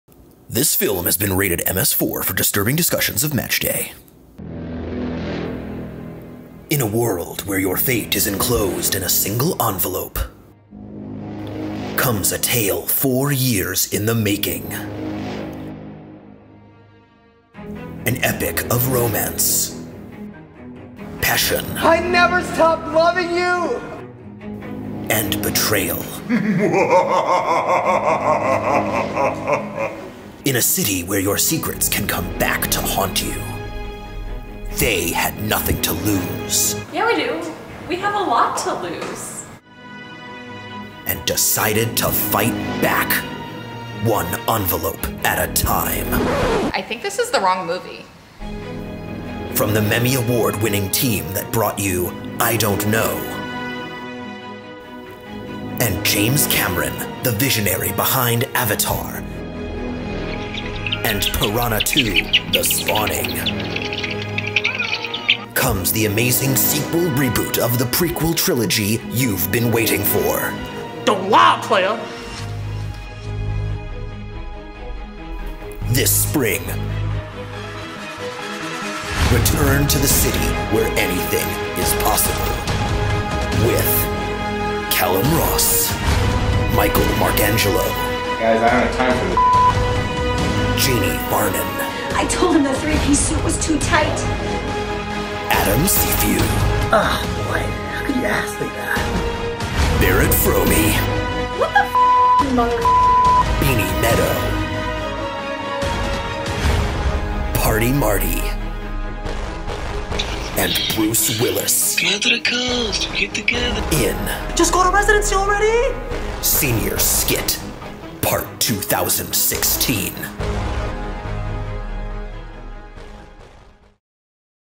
Senior Skit: 2016 Trailer
Trailer for Senior Skit:2016 by the Pritzker School of Medicine Class of 2016

Script and Voice Over: Party Marty
Editing: Omar Malas